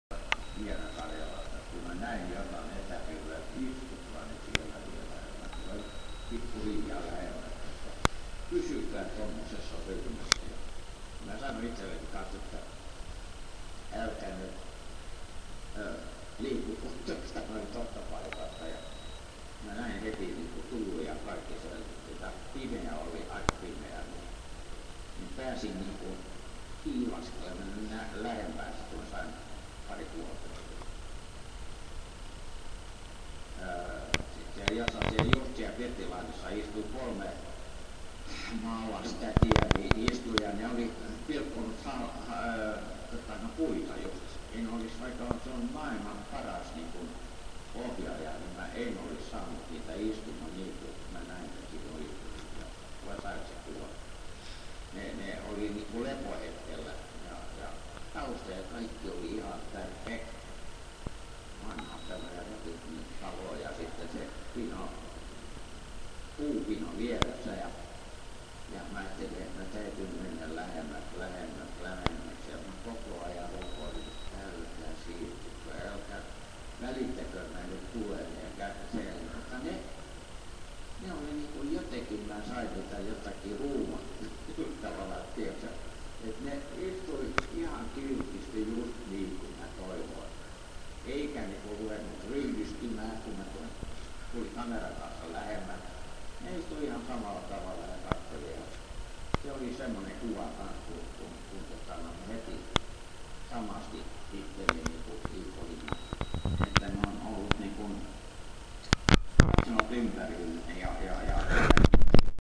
20120415P4150046.MP4
Caj Bremer-näyttely. Näyttelyvideossa kuvaaja kertoo nuorten naisten kuvasta ja puunhakkaajanaisten taukohetkestä.